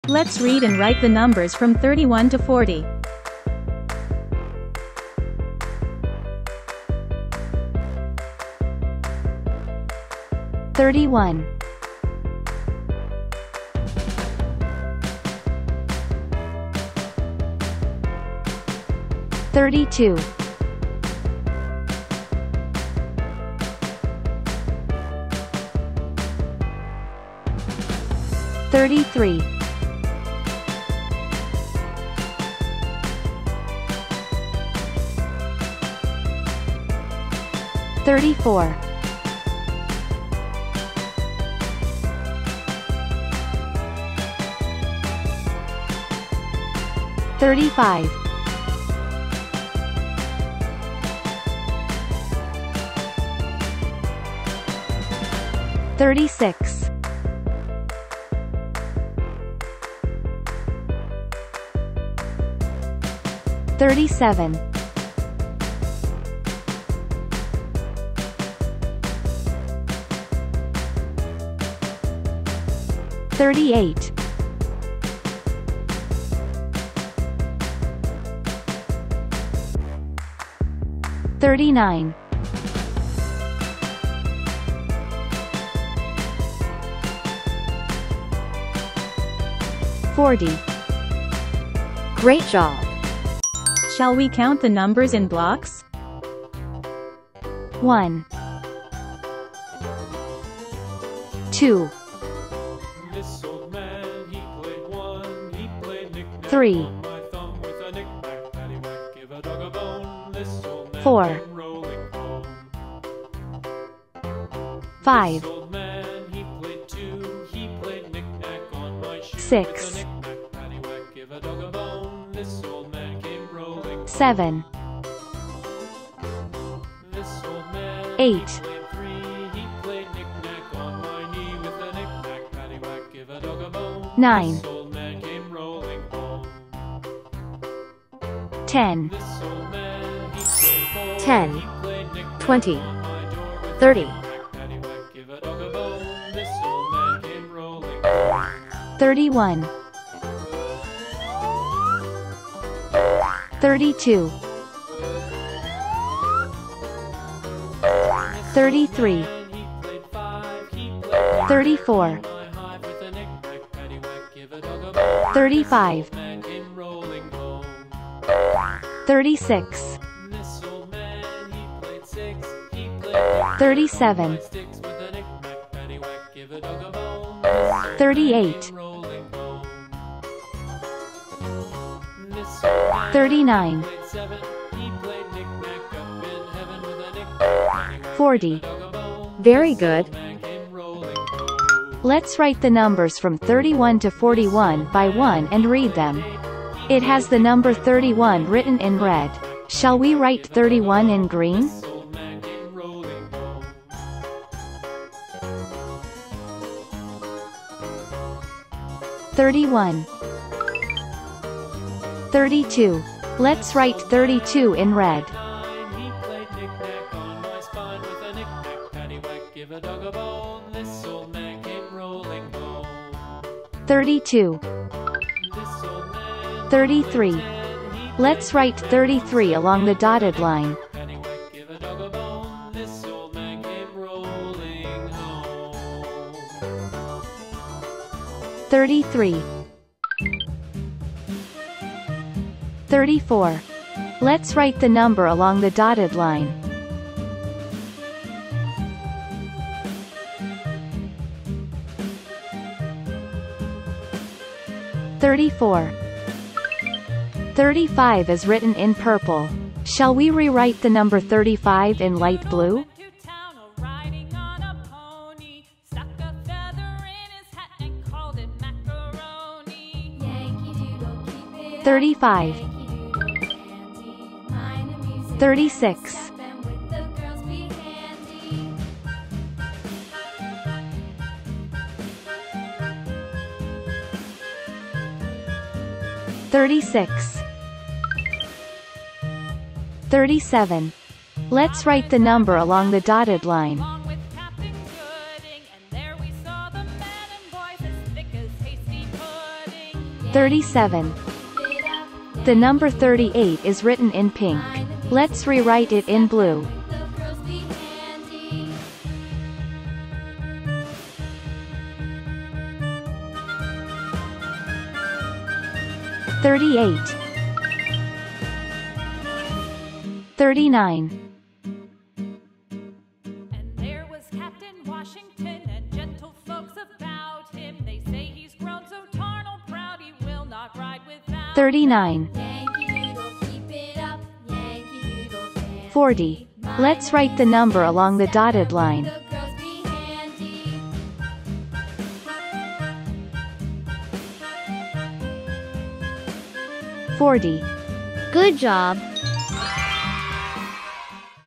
[ABC for Kids] Learn numbers 31-40 for kids | how to write numbers
Hi guys!

learn numbers!
Read the numbers as you write the numbers from 31 to 40 along the dotted line.
Learn to count to 40 while counting blocks.